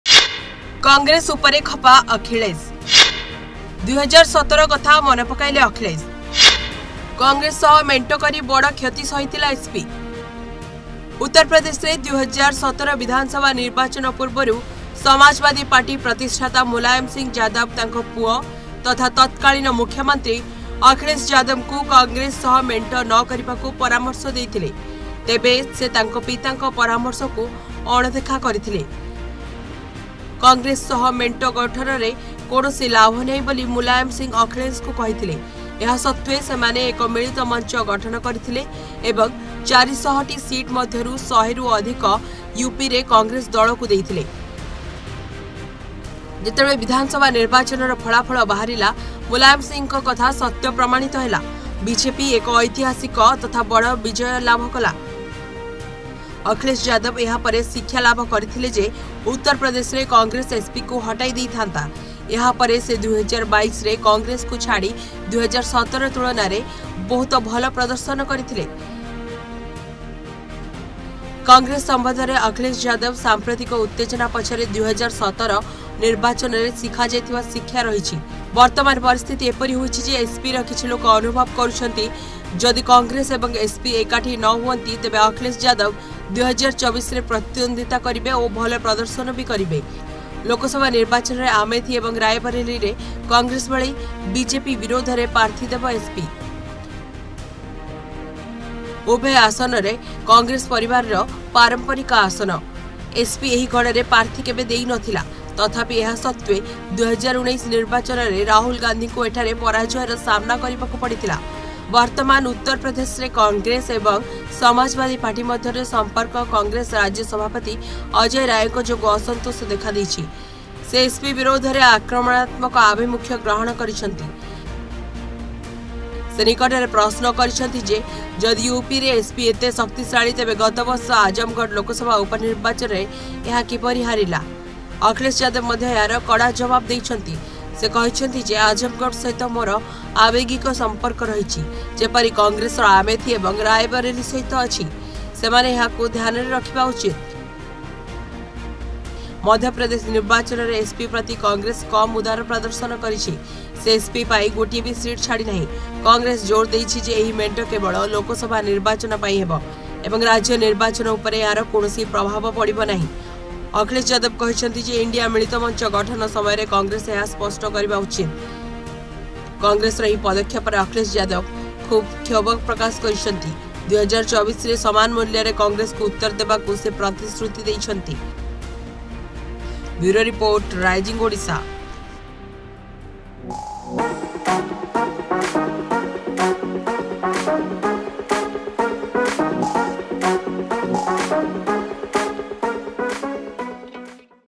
When Akhilesh Yadav Didn't Listen to His Father Mulayam Yadav in 2017 UP Election! | SP Vs Congress
When Akhilesh Yadav Didn't Listen to His Father Mulayam Yadav in 2017 UP Election!